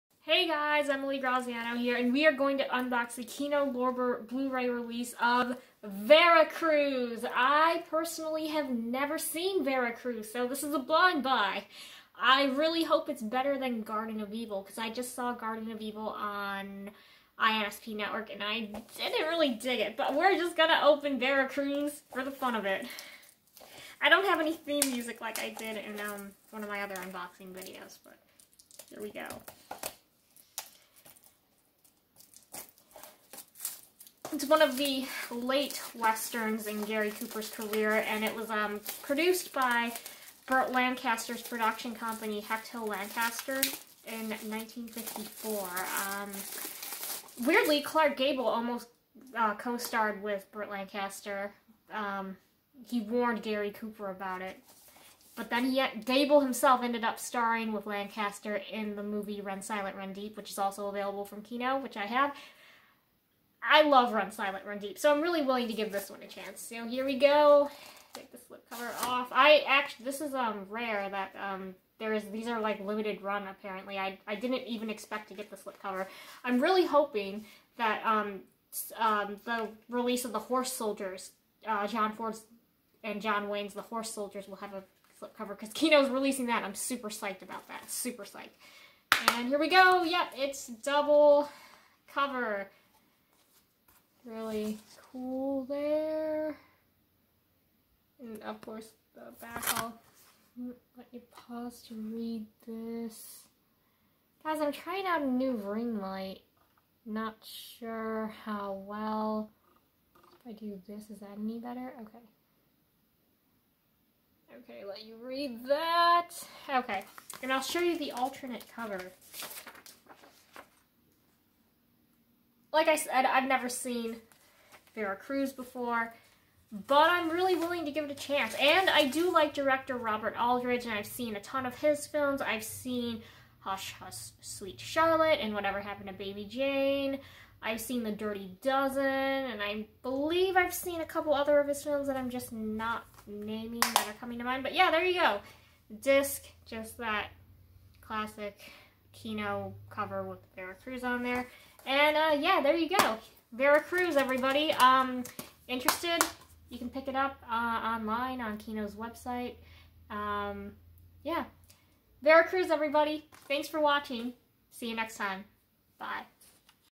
Vera Cruz 1954: Kino Lorber blu ray unboxing
Join me as I unbox the Gary Cooper and Burt Lancaster 1954 western: Vera Cruz!